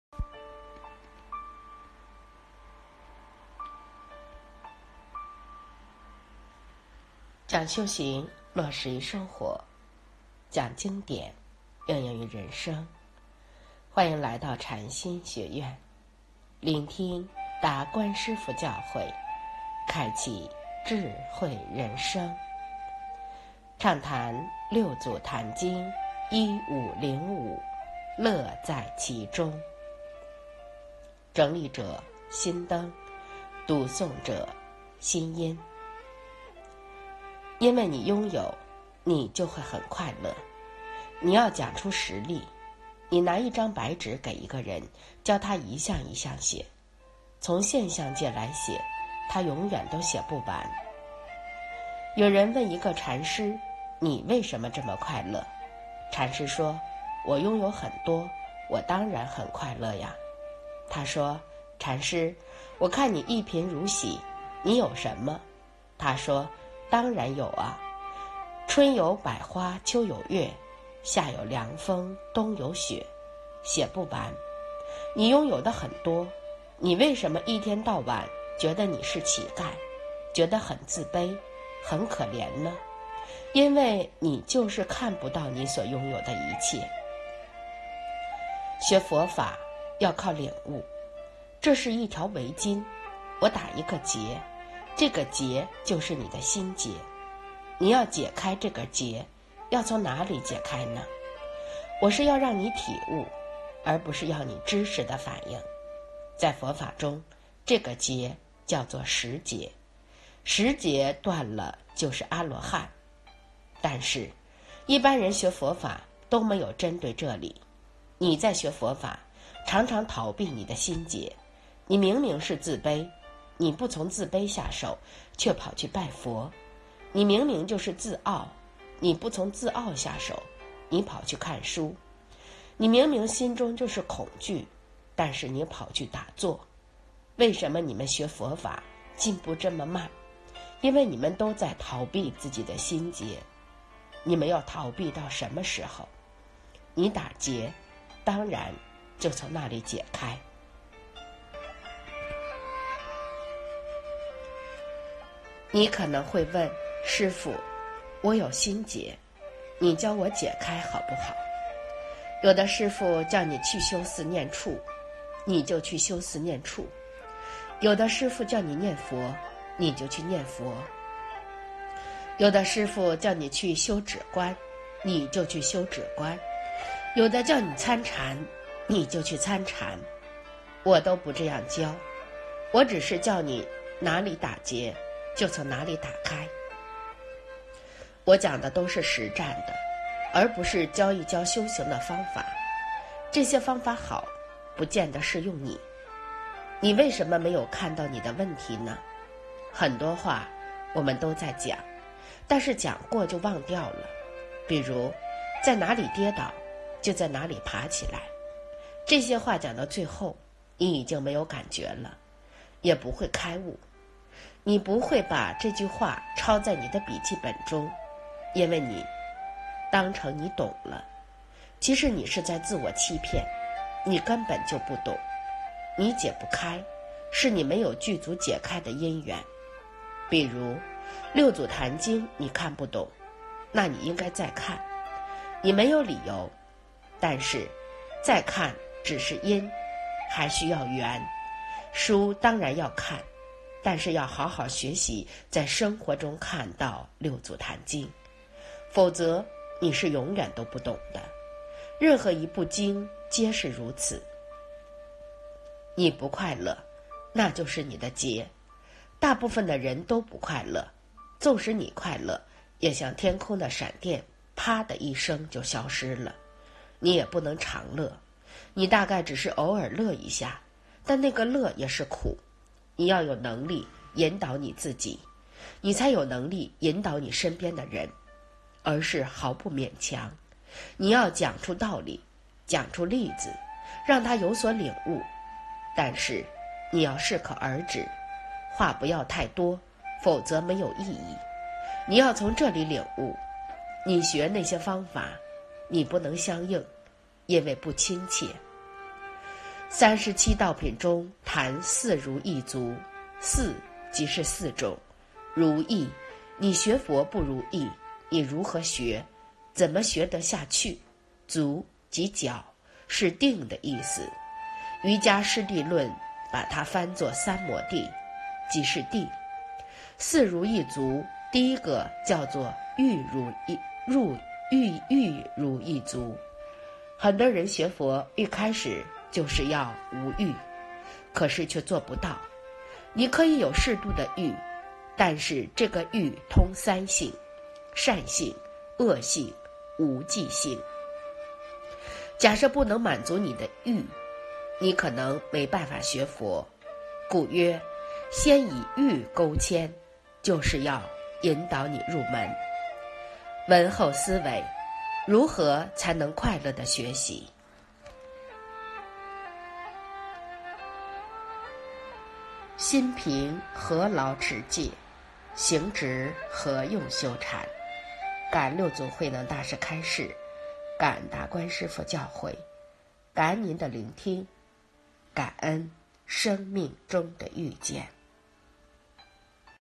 暢談六祖壇經（1505）樂在其中
因為你擁有，你就會很快樂。你要講出實例，你拿一張白紙給一個人，叫他一項一項寫，從現象界來寫，他永遠都寫不完。有人問一個禪師：「你為什麼這麼快樂？」禪師說：「我擁有很多，我當然很快樂啊！」他說：「禪師，我看你一貧如洗，你有什麼？」他說：「當然有啊！春有百花秋有月，夏有涼風冬有雪。」寫不完，你擁有的很多，你為什麼一天到晚覺得你是乞丐，覺得很自卑、很可憐呢？因為你就是看不到你所擁有的一切。

　　學佛法要靠領悟。這是一條圍巾，我打一個結，這個結就是你的心結。你要解開這個結，要從哪裡解開呢？我是要讓你體悟，而不是要你知識的反應。在佛法中，這個結叫做十結，十結斷了就是阿羅漢。但是，一般人學佛法都沒有針對這裡。你在學佛法，常常逃避你的心結。你明明是自卑，你不從自卑下手，卻跑去拜佛；你明明就是自傲，你不從自傲下手，你跑去看書；你明明心中就是恐懼，但是你跑去打坐。為什麼你們學佛法進步這麼慢？因為你們都在逃避自己的心結。你們要逃避到什麼時候？你打結，當然就從那裡解開。

　　你可能會問：「師父，我有心結，你教我解開好不好？」有的師父叫你去修四念處，你就去修四念處；有的師父叫你念佛，你就去念佛；有的師父叫你去修止觀，你就去修止觀；有的叫你參禪，你就去參禪。我都不這樣教，我只是叫你哪裡打結，就從哪裡打開。我講的都是實戰的，而不是教一教修行的方法。這些方法好，不見得適用你。

　　你為什麼沒有看到你的問題呢？很多話我們都在講，但是講過就忘掉了，比如：「在哪裡跌倒，就在哪裡爬起來。」這些話講到最後，你已經沒有感覺了，也不會開悟。你不會把這句話抄在你的筆記本中，因為你當成你懂了，其實你是在自我欺騙，你根本就不懂。你解不開，是你沒有具足解開的因緣。比如，《六祖壇經》你看不懂，那你應該再看，你沒有理由。但是，再看只是因，還需要緣，書當然要看，但是要好好學習在生活中看到《六祖壇經》，否則你是永遠都不懂的。任何一部經皆是如此。

　　你不快樂，那就是你的結。大部份的人都不快樂。縱使你快樂，也像天空的閃電，啪地一聲就消失了，你也不能常樂，你大概只是偶爾樂一下，但那個樂也是苦。你要有能力引導你自己，你才有能力引導你身邊的人，而是毫不勉強，你要講出道理，講出例子，讓他有所領悟。但是，你要適可而止，話不要太多，否則沒有意義。你要從這裡領悟。你學那些方法，你不能相應，因為不親切。

　　《三十七道品》中談四如意足。「四」即是四種；「如意」，你學佛不如意，你如何學？怎麼學得下去？「足」即腳，是定的意思。《瑜伽師地論》把它翻作三摩地，即是定。

　　四如意足，第一個叫做欲如意足。很多人學佛一開始就要無欲，可是卻做不到。你可以有適度的欲，但是這個欲通三性：善性、惡性、無際性。假設不能滿足你的欲，你可能沒辦法學佛，故曰「先以欲鉤牽」，就是要引導你入門。

聞後思惟：如何才能快樂地學習？